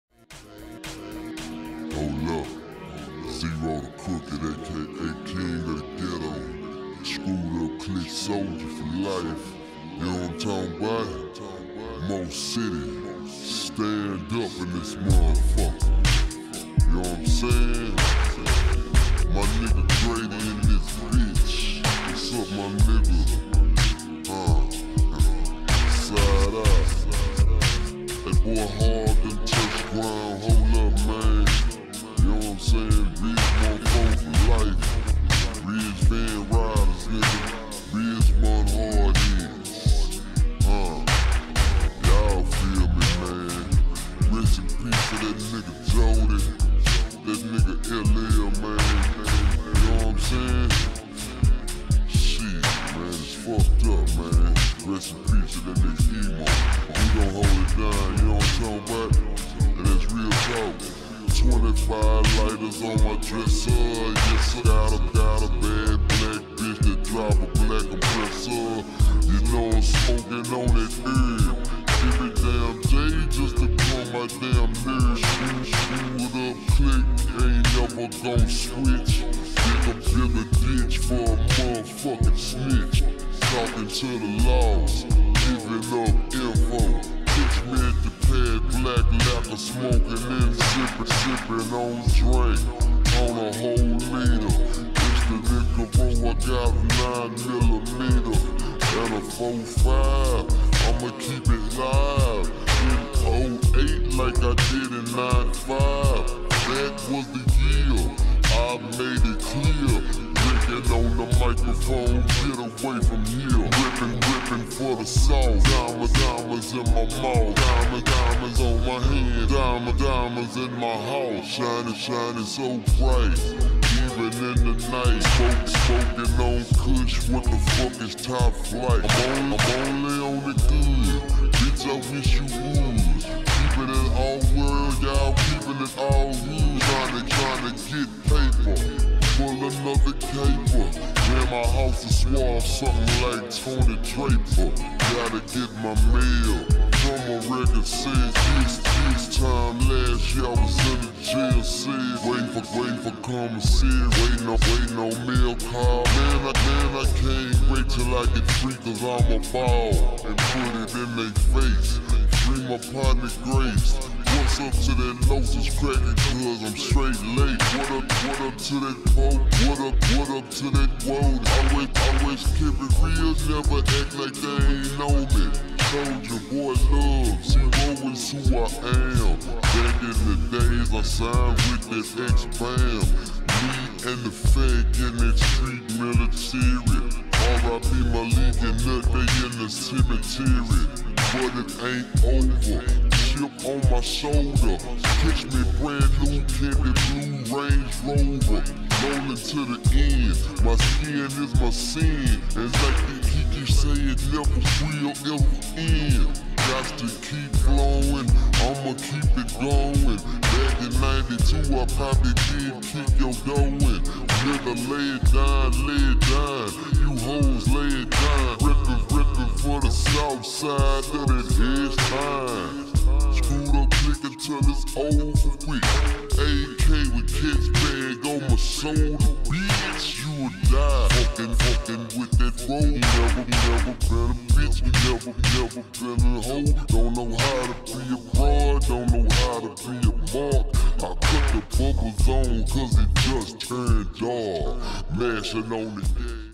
Charged_motorworks bumper herculeds all the lights Tiarra Grilles all the custom grilles 6651custom
06 Daytona rt custom bumper,lights & grilles this video is on slow mo !